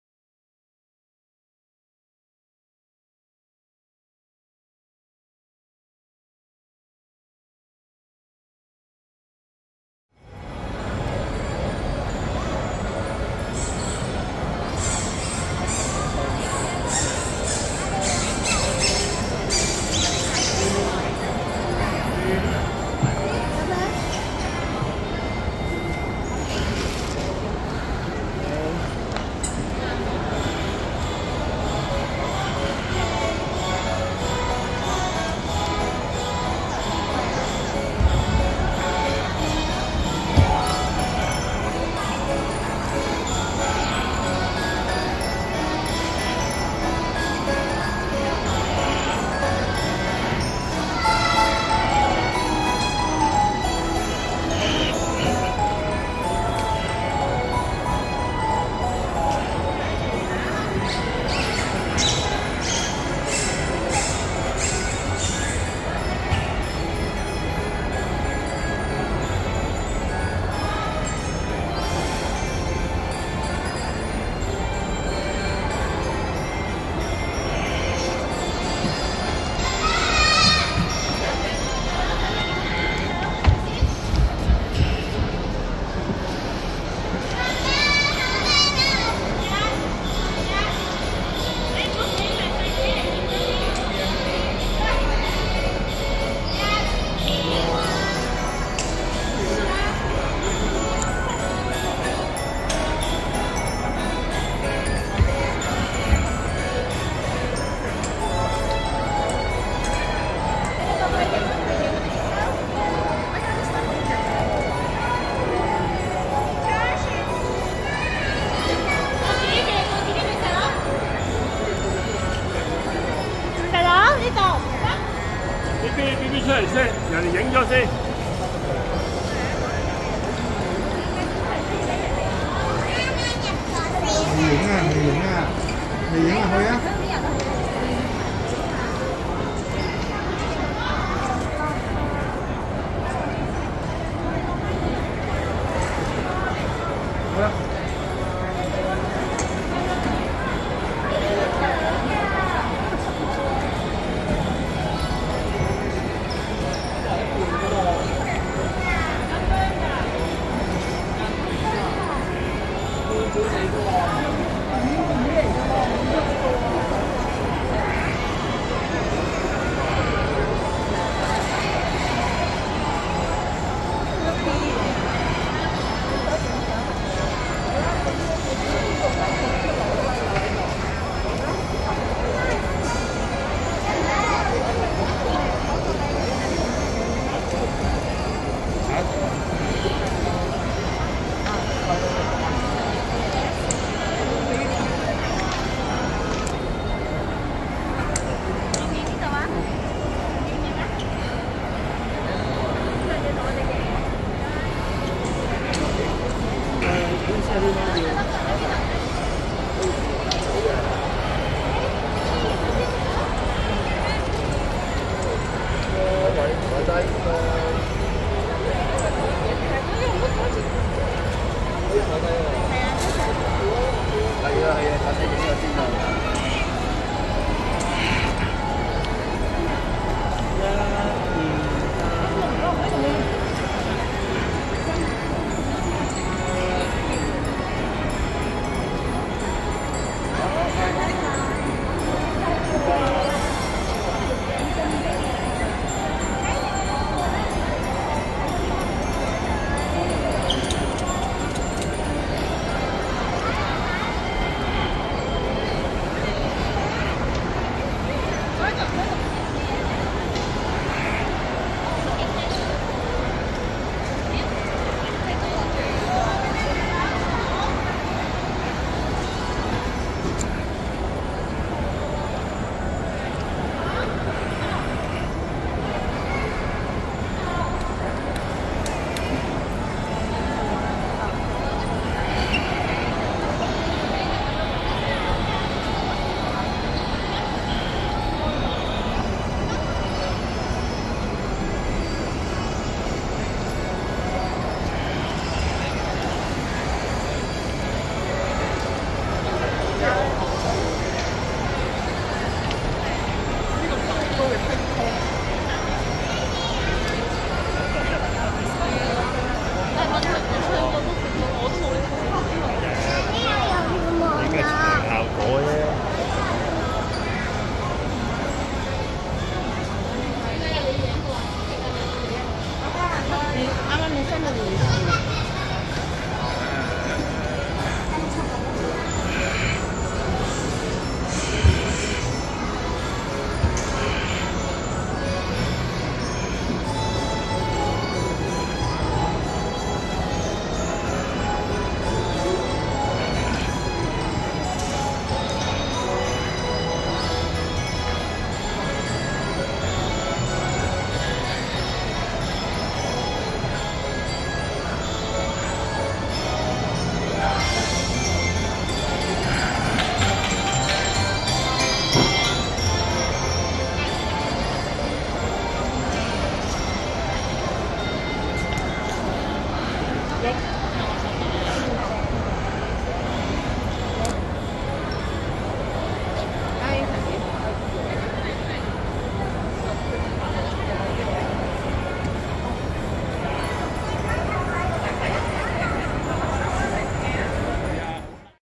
「『Joy to the World of Van Gogh』梵高沉浸式體驗展 x 聖誕大型裝置」@MegaBox - 2022年11月20日
錄影日期:2022年11月20日
錄影器材:Samsung Galaxy S22+ 5G
剪輯軟件:iMovie (Apple macOS)
影片解像度:1920x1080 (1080p,Full High Definition)
影片長度:6分0秒